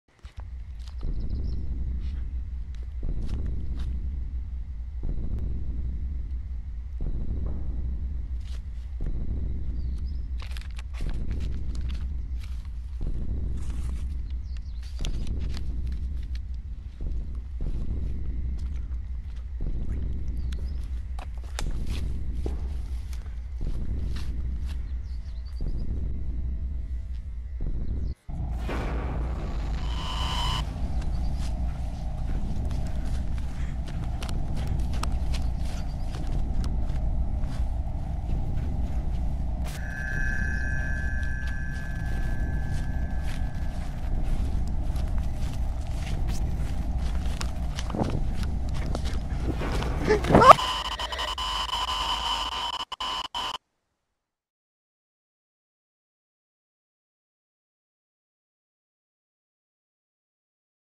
Slender In Real Life
This is footage that was found in the woods, this is just a preview of what was found